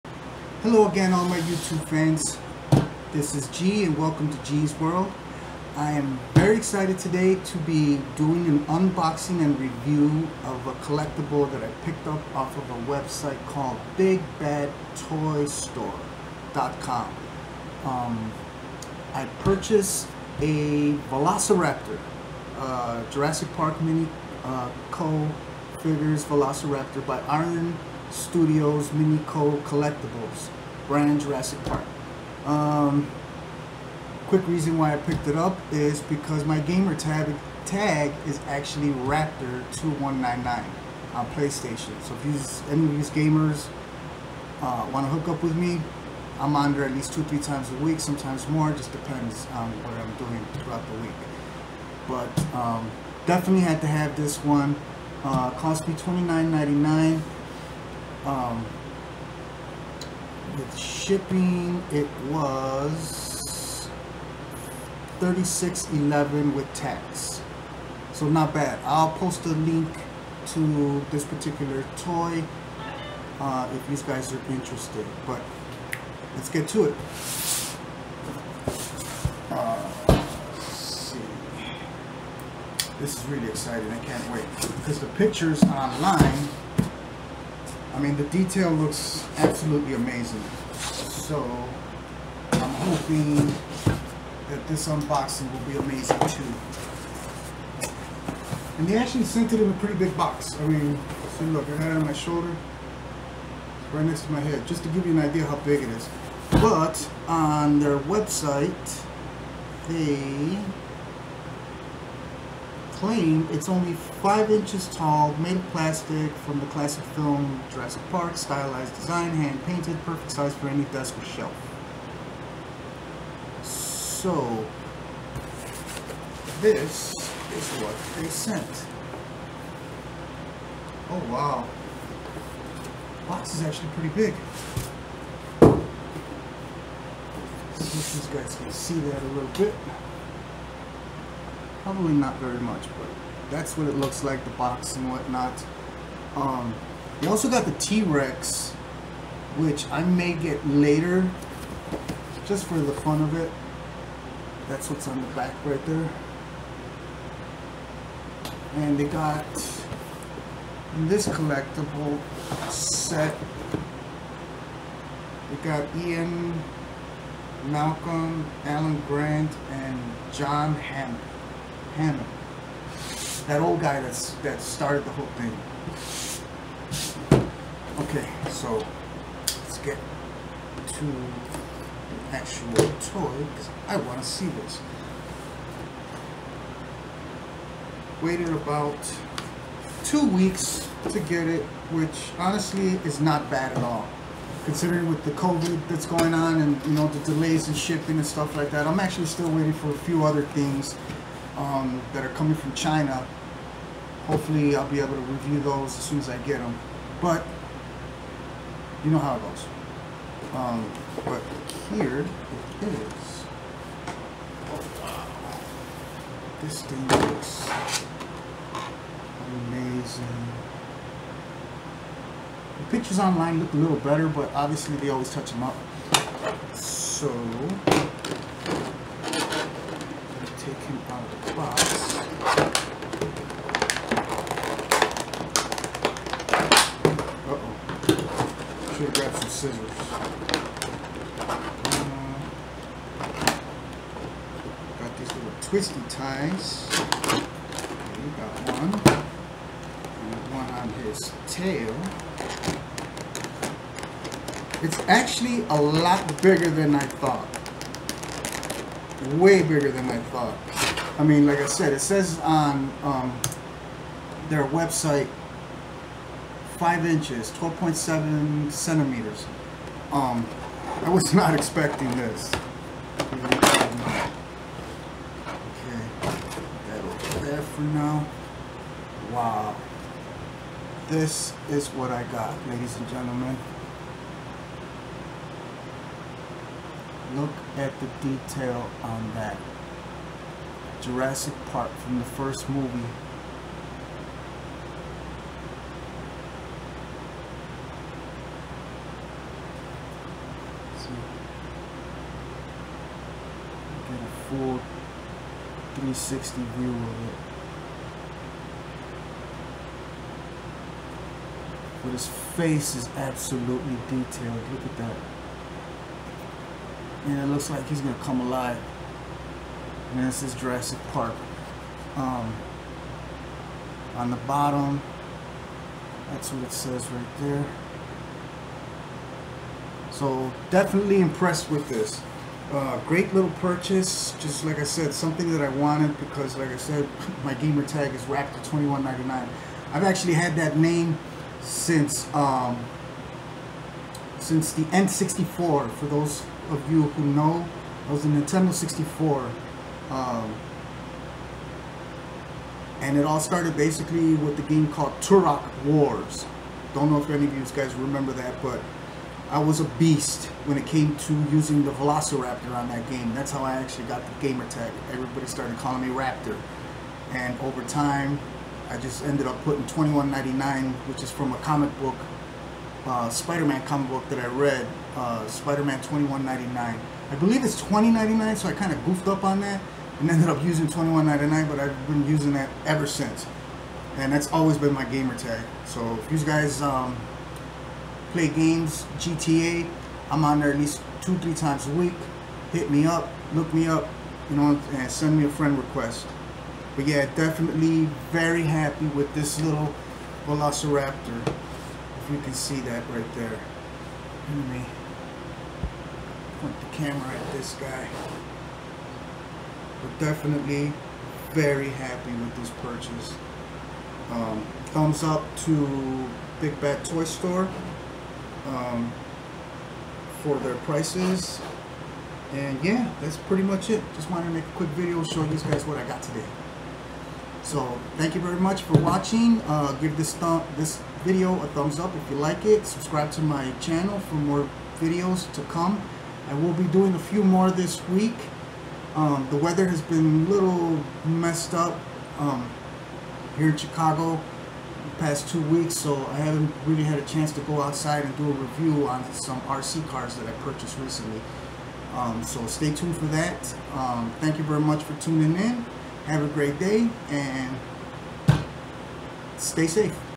Jurassic Park Velociraptor Unboxing & Review
This a quick review of a Mini Co. figurine of a Velociraptor from the first movie Jurassic Park by: Iron Studios, Mini Co. Collectibles
Purchased from Big Bad Toy Store